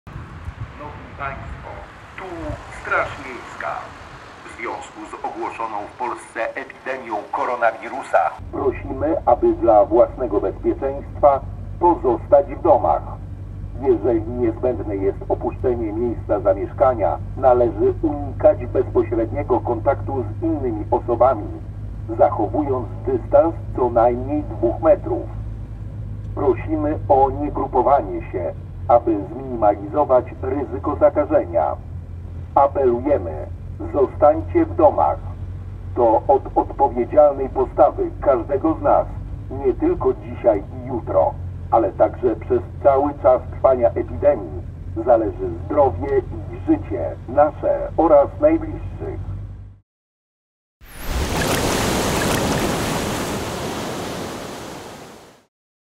Głośne zostańwdomu na ulicach Gdyni
„Apelujemy, zostańcie w domach. To od odpowiedzialnej postawy każdego z nas, nie tylko dzisiaj i jutro, ale także przez cały czas trwania epidemii, zależy zdrowie i życie nasze oraz najbliższych” – komunikaty takiej treści, odtwarzane z radiowozów Straży Miejskiej, można od dzisiaj usłyszeć na ulicach Gdyni. To kolejna inicjatywa miasta realizowana w trosce o bezpieczeństwo mieszkańców. Wkrótce do tych działań prewencyjnych dołączy też gdyńska policja.